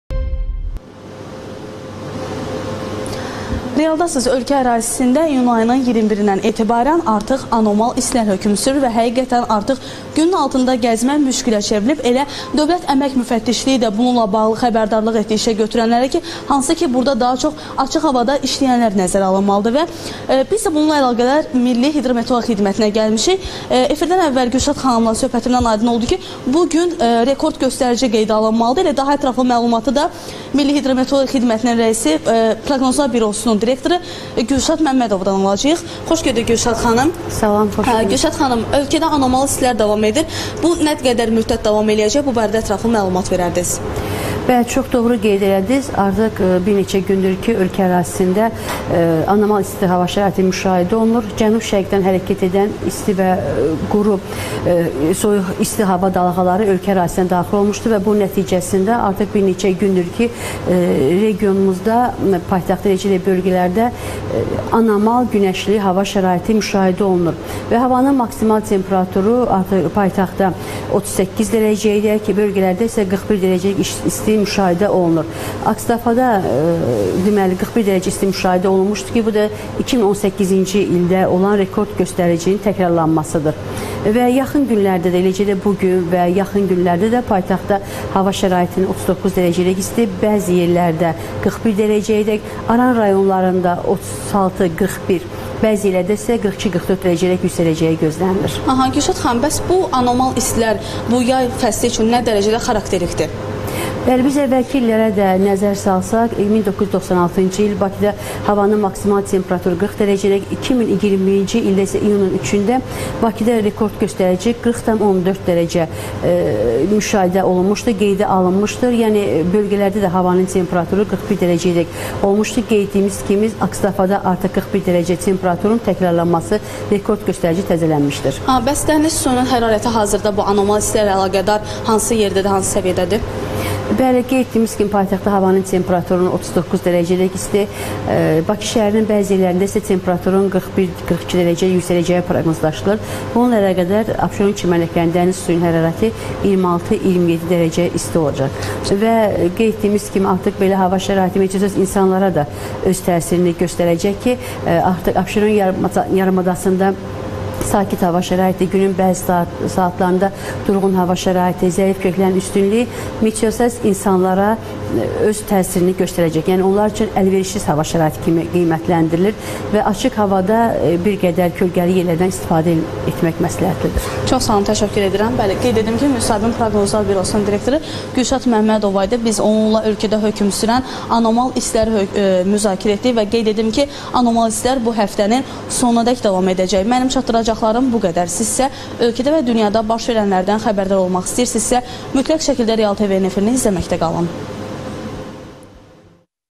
Anomal hava şəraiti hökm sürür
Bu gün 44 dərəcə rekord temperaturun qeydə alınacağı gözlənilir